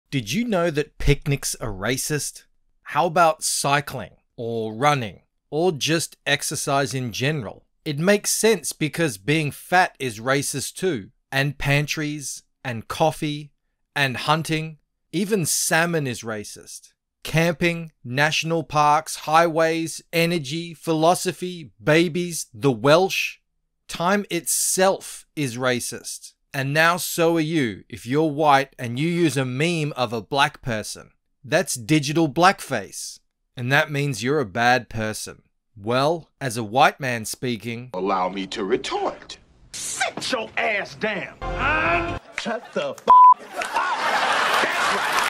DIGITAL BLACKFACE! And all the reasons you're a bad person
What’s ‘digital blackface?’ And why is it wrong when White people use it?

Digital blackface is a practice where White people co-opt online expressions of Black imagery, slang, catchphrases or culture to convey comic relief or express emotions.

These expressions, what one commentator calls racialized reactions, are mainstays in Twitter feeds, TikTok videos and Instagram reels, and are among the most popular Internet memes.

Digital blackface involves White people play-acting at being Black, says Lauren Michele Jackson, an author and cultural critic, in an essay for Teen Vogue. Jackson says the Internet thrives on White people laughing at exaggerated displays of Blackness, reflecting a tendency among some to see “Black people as walking hyperbole.”

If you’re still not sure how to define digital blackface, Jackson offers a guide. She says it “includes displays of emotion stereotyped as excessive: so happy, so sassy, so ghetto, so loud… our dial is on 10 all the time — rarely are black characters afforded subtle traits or feelings.”

Many White people choose images of Black people when it comes to expressing exaggerated emotions on social media – a burden that Black people didn’t ask for, she says.

“We are your sass, your nonchalance, your fury, your delight, your annoyance, your happy dance, your diva, your shade, your ‘yaas’ moments,” Jackson writes. “The weight of reaction GIFing, period, rests on our shoulders.”

Why digital blackface is wrong

Some may say posting a video of Sweet Brown saying, “Oh Lord Jesus, it’s a fire” is just for laughs. Why overthink it? Why give people yet another excuse for labeling White people racists for the most innocuous behaviors?

But critics say digital blackface is wrong because it’s a modern-day repackaging of minstrel shows, a racist form of entertainment popular in the 19th century. That’s when White actors, faces darkened with burnt cork, entertained audiences by playing Black characters as bumbling, happy-go-lucky simpletons. That practice continued in the 20th century on hit radio shows such as “Amos ‘n’ Andy.”

Put simply: digital blackface is 21st-century minstrelsy.

“Historical blackface has never truly ended, and Americans have yet to actively confront their racist past to this day,” Erinn Wong writes in an academic paper on the topic.

“In fact, minstrel blackface has emerged into even more subtle forms of racism that are now glorified all over the Internet.”

Wong says that digital blackface is wrong because it “culturally appropriates the language and expressions of black people for entertainment, while dismissing the severity of everyday instances of racism black people encounter, such as police brutality, job discrimination, and educational inequity.”